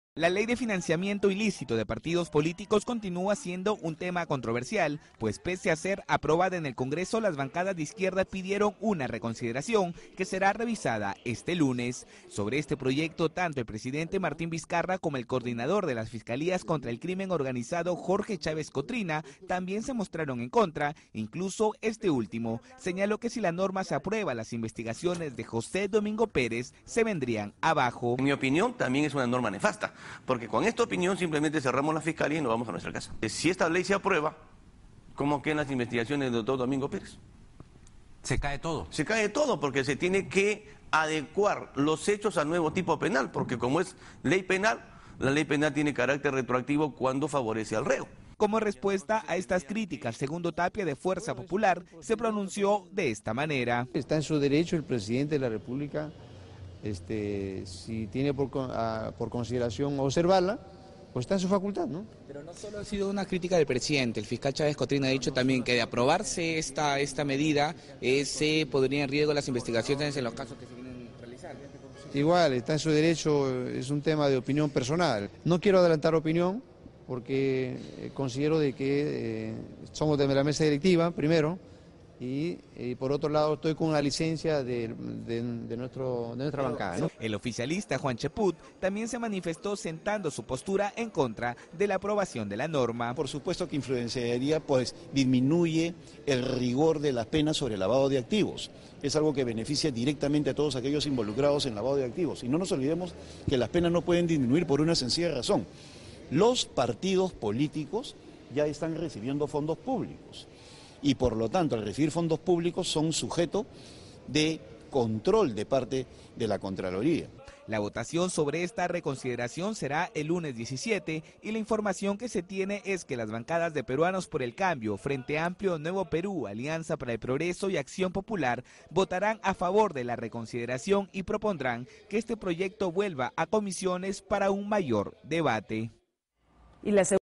Cinco bancadas a favor de reconsideración de ley de partidos políticos
La ley de financiamiento ilícito de partidos políticos continúa siendo un tema controversial, pues pese a ser aprobada en el Congreso, las bancadas de izquierda pidieron una reconsideración que será revisada este lunes.

Sobre este proyecto, tanto el presidente Martín Vizcarra como el coordinador de las fiscalías contra el crimen organizado, Jorge Chávez Cotrina, también se mostraron en contra, incluso este último señaló que si la norma se aprueba, las investigaciones de José Domingo Pérez se vendrían abajo.

Como respuesta a estas críticas Segundo Tapia del partido Fuerza Popular se pronunció de esta manera: “Está en su derecho el presidente de la República, tiene por consiguiente observar la extensa facultad, pero no sólo el presidente sino también el fiscal Chávez Cotrina”, afirmó. Por su parte, el oficialista Juan Sheput también se pronunció sentando su postura en contra de la aprobación de norma.

Vale mencionar que la votación sobre esta reconsideración se llevará a cabo este lunes 17. La información que se maneja es que las bancadas de Peruanos por el Kambio (PPK), Frente Amplio, Nuevo Perú, Alianza para el Progreso y Acción Popular votarán a favor de la reconsideración y propondrán que este proyecto vuelva a comisiones para un mayor debate.